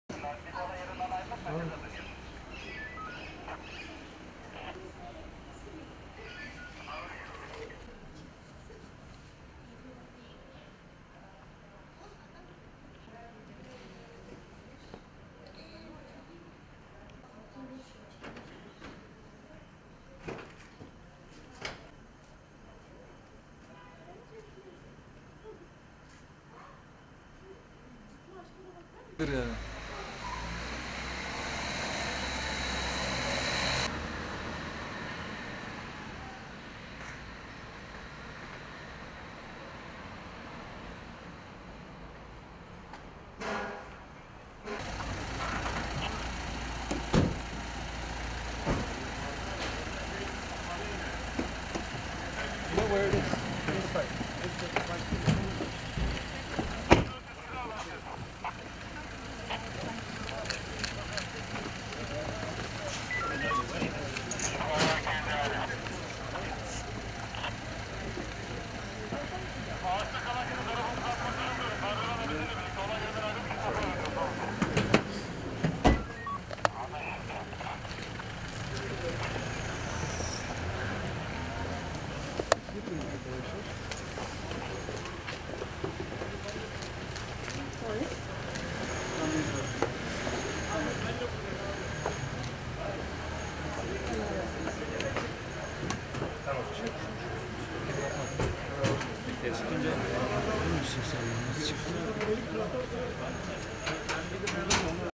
BM Raportörü Callamard STK temsilcileriyle görüşüyor
Gazeteci Cemal Kaşıkçı cinayeti soruşturması kapsamında Türkiye'ye gelen Birleşmiş Milletler (BM) raportürü Callamard ve beraberindekiler, Türk Arap Medya Derneği'nde STK temsilcileriyle bir araya geldi.

BM Yargısız ve Keyfi İnfazlar Özel Raportörü Agnes Callamard, İngiliz avukat Helena Kennedy ile eski Dünya Adli Tıp Akademisi Başkanı ve Coimbra Üniversitesi Tıp Fakültesi Müdürü Prof. Dr. Duarte Nuno Vieira'nın bulunduğu heyet, Fatih'te bulunan Türk Arap Medya Derneği'ne geldi. Heyet'in burada Dernek Başkanı Turan Kışlakçı ile bazı STK temsilcileriyle görüşecekleri öğrenildi.